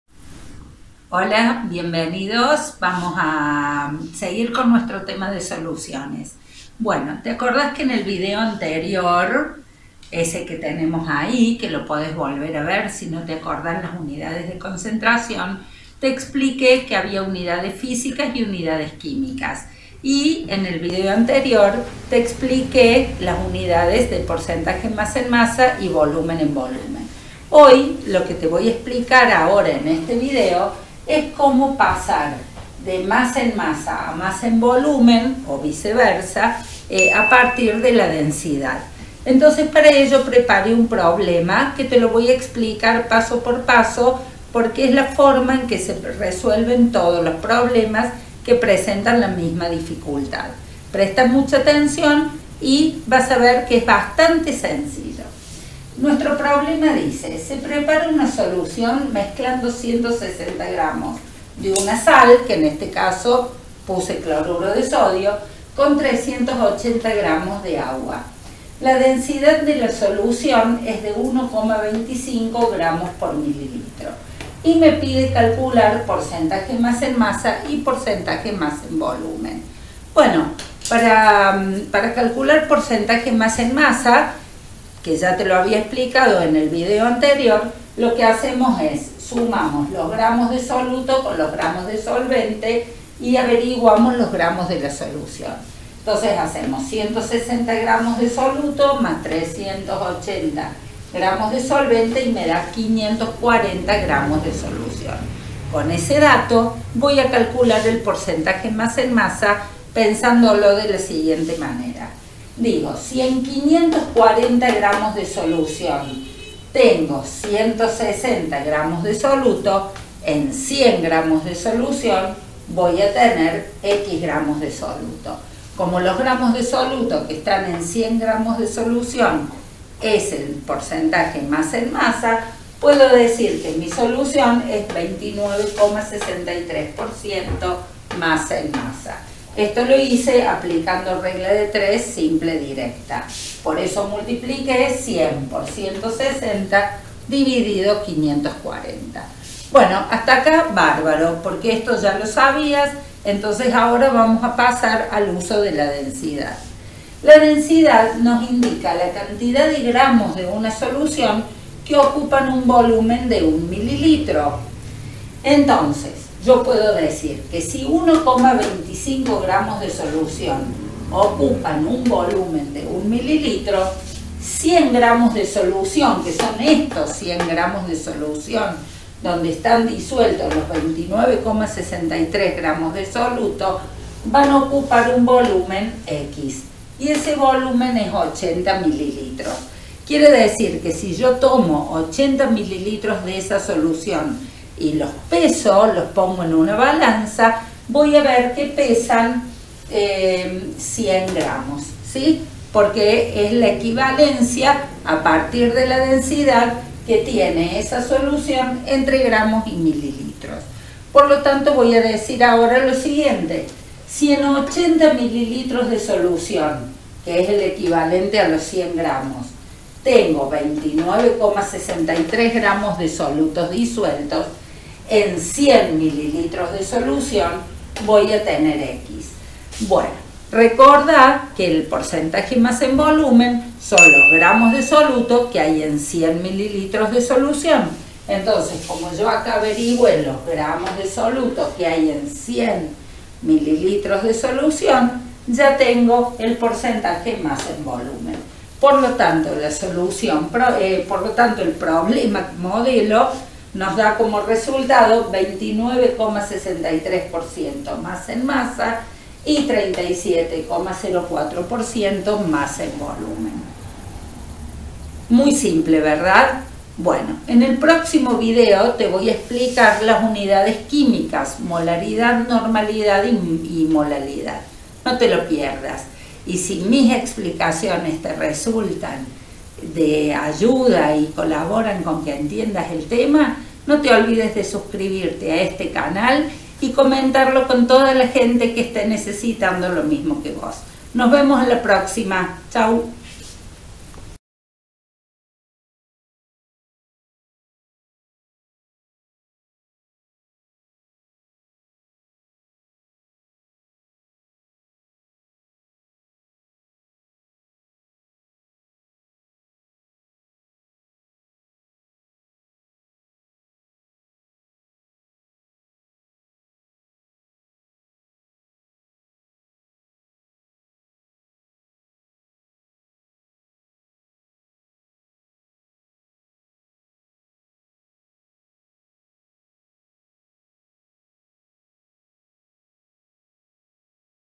Soluciones quimicas 2. . Densidad, %m/m, %m/v. Pasaje de masa de solucion a volumen y viceversa.
Teoría y problemas para aprender a usar la densidad como herramienta  para obtener la masa  o peso de una solucion a partir de su volumen, o para obtener el volumen a partir de la masa. Problema modelo explicado paso a paso.